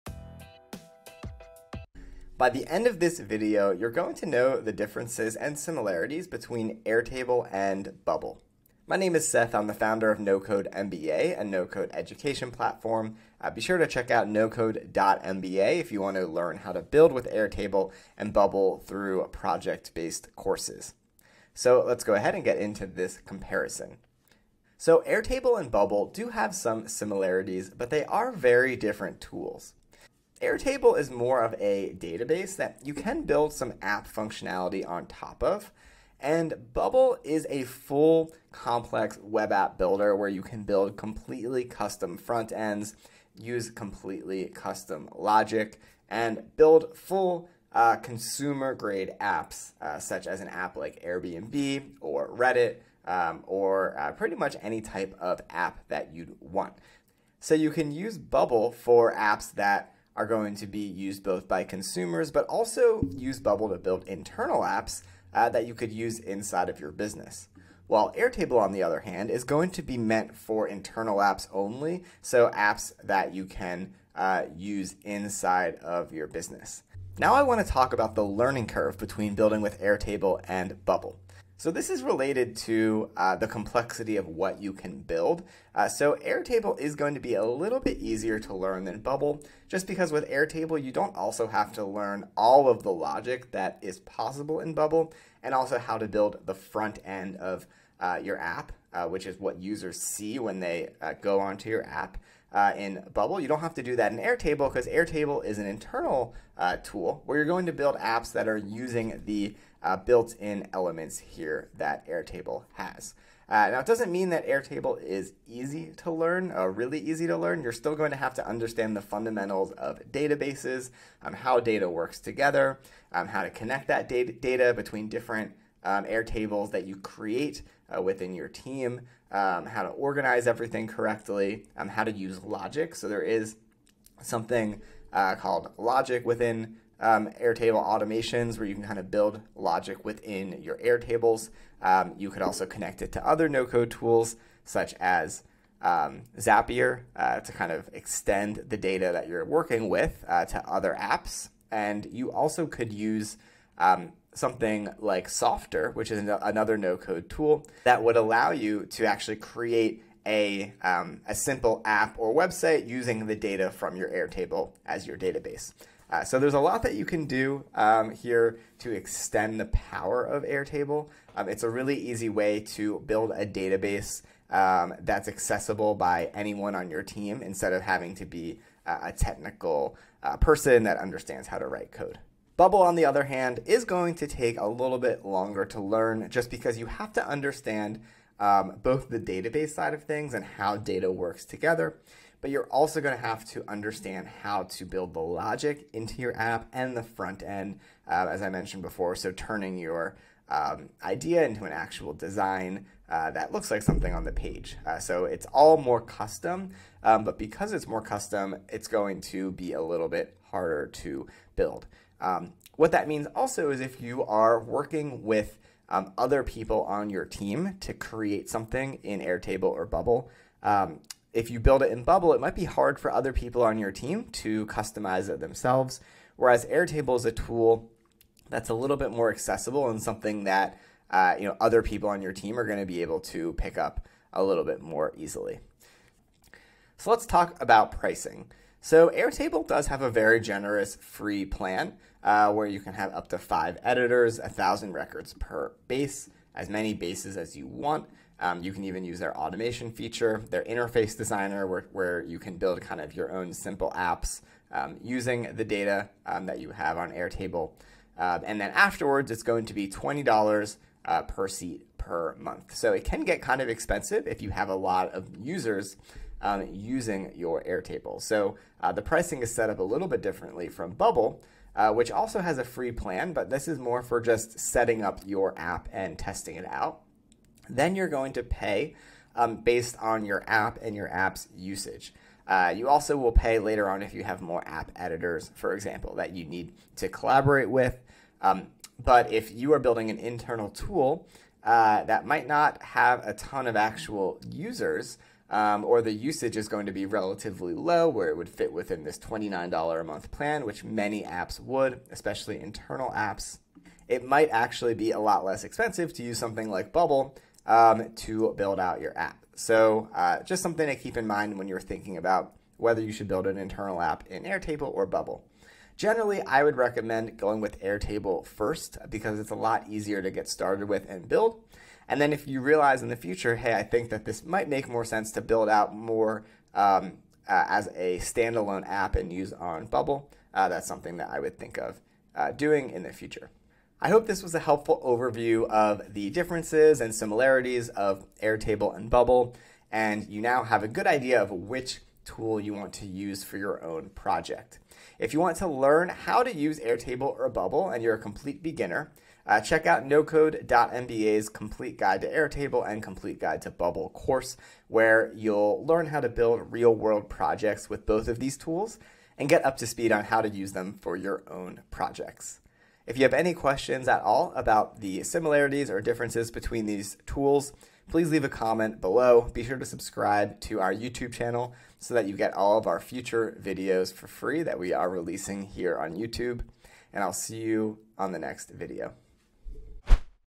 Airtable vs Bubble: know this key difference before building your app (+ pricing explained)
Airtable and Bubble. Two popular tools in the no code space. But which one should you use?

We're going unravel the nuances of Airtable's user-friendly database functionality against Bubble's comprehensive web app building capabilities.

Discover how each platform caters to different needs: Airtable for internal business tools with its intuitive database management, and Bubble for crafting intricate, consumer-grade applications. We're breaking down the learning curves, usability, and pricing strategies to help you decide which tool best fits your project.

Whether you're building for internal purposes or aiming for the next big consumer app, this comparison will shed light on the right choice for your venture. Don't miss out on key insights that could shape the future of your project. Watch now to find your ideal no-code solution.

⭐ TAKE OUR FULL COURSE ON BUILDING APPS USING AI